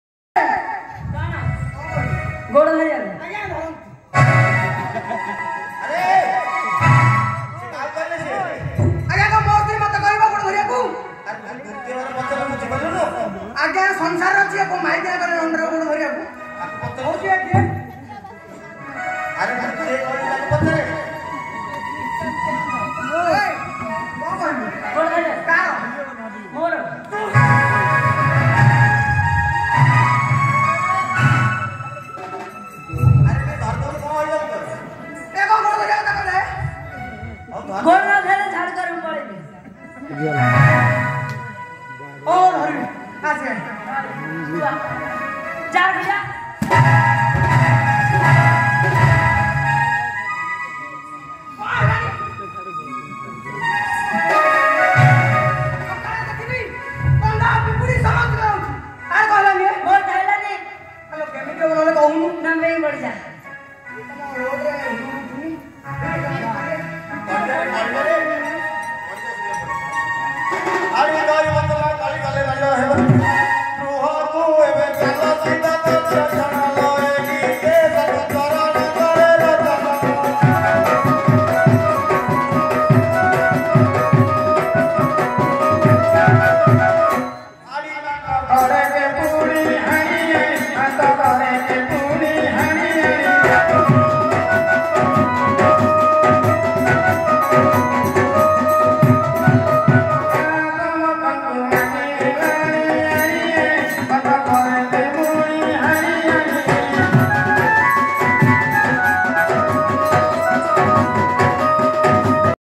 pathara danda nacha 2024 | hadi hadiani comedy
ଦଣ୍ଡ ନାଚ ଓଡିଶାର ଏକ ପ୍ରାଚୀନ ଓ ପାରମ୍ପରିକ ନାଚ ! ଏହା ଗଞ୍ଜାମ ଜିଲ୍ଲାର ଏକ ଲୋକ ପର୍ବ  ! ଏହା ଗ୍ରାମାଞ୍ଚରେ କାମନା ଦଣ୍ଡ ଭାବେ ପରିଚୟ